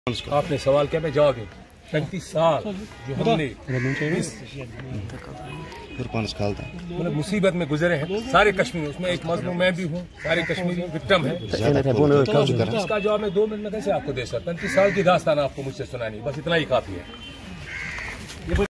Atrocities of 35 years can't be summed up in two minutes: Afzal Guru's brother
Atrocities of 35 years can't be summed up in two minutes, says Afzal Guru's brother Ajaz Guru as he files nomination from Sopore.